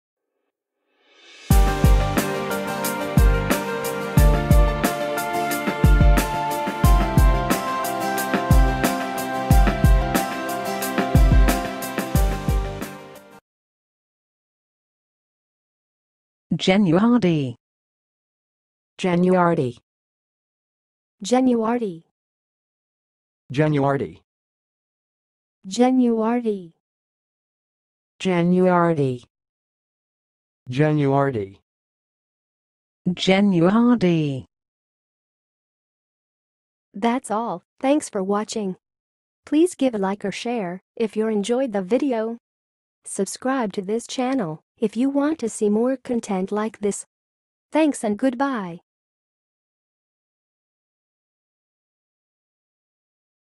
GENUARDI - How to pronounce it?
GENUARDI - Say Genuardi like an expert: Pronunciation guide in 2023

Genuardi was a chain of supermarkets located primarily in Pennsylvania and New Jersey.
Founded in 1920, the company built a reputation for providing high-quality products and friendly service.
Genuardi's stores offered a wide range of grocery items, including fresh produce, meats, dairy products, and pantry staples.
In addition to groceries, many Genuardi's locations also featured pharmacies, bakeries, and delis.
The company prided itself on being a trusted neighborhood grocery store where customers could find everything they needed for their daily shopping.

Pronunciation:
To pronounce Genuardi, you can say juh-nwahr-dee.
Alternative variants include juh-ner-dee or juh-noo-ar-dee.

Test yourself by speaking the following examples:
1. I went to Genuardi to buy some fresh produce and pantry staples
2. The Genuardi store in my neighborhood has a great bakery
3. Genuardi was known for its high-quality products and friendly service
4. Many people relied on Genuardi for their daily grocery shopping